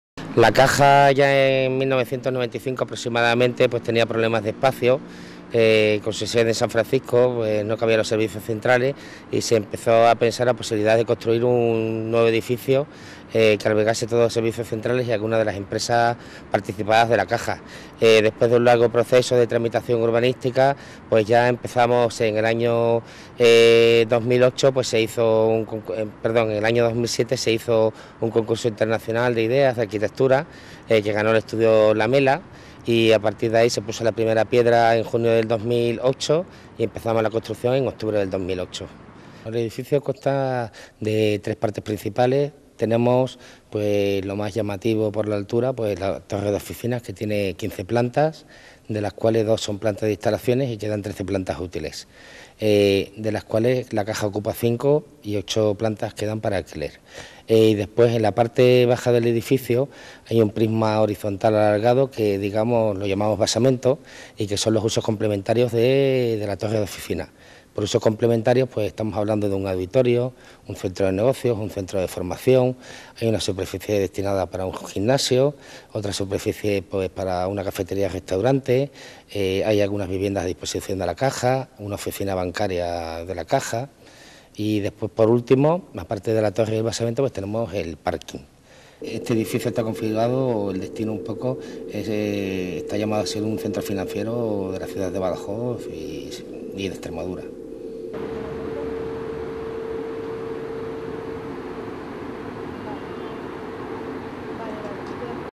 Nuevo edificio Caja Badajoz en Extremadura
El jefe del Departamento de Implantación del Nuevo Edificio de Caja de Badajoz, Javier Andrada, ha destacado que 'Badajoz siglo XXI' "está llamado a ser un centro financiero" de la capital pacense y de Extremadura.